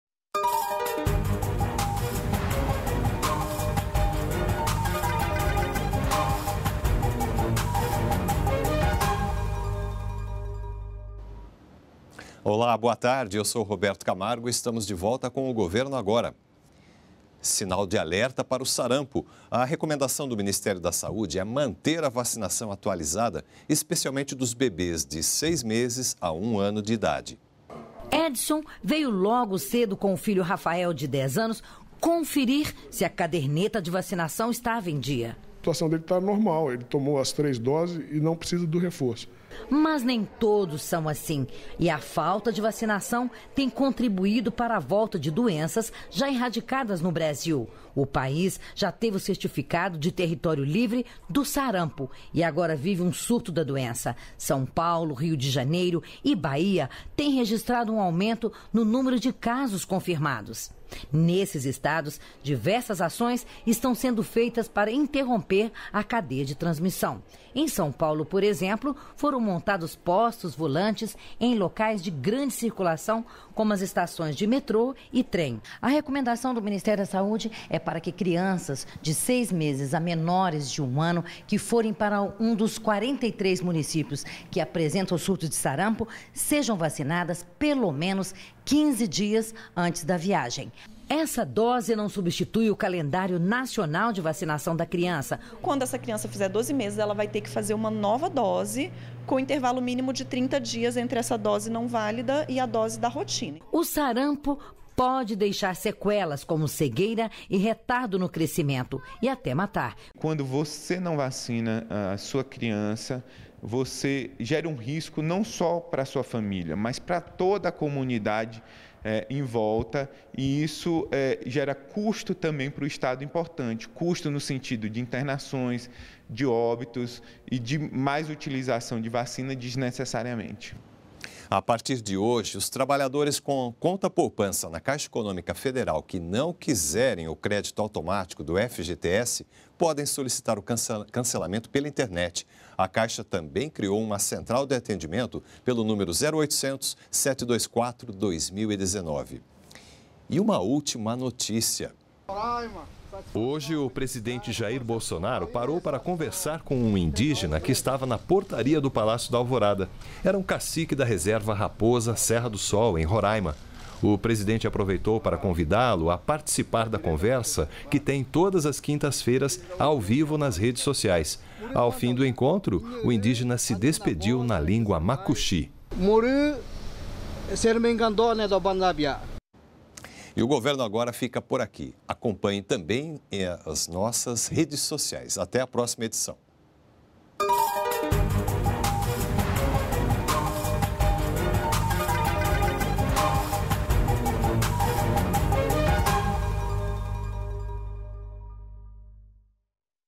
Ministério da Saúde alerta contra o Sarampo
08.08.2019 - O Ministério da Saúde recomenda manter a vacinação contra o Sarampo atualizada, especialmente nos bebês de seis meses a um ano de idade. E hoje o presidente Bolsonaro parou para conversar com o Cacique da reserva Raposa-Serra do Sol, em Roraima.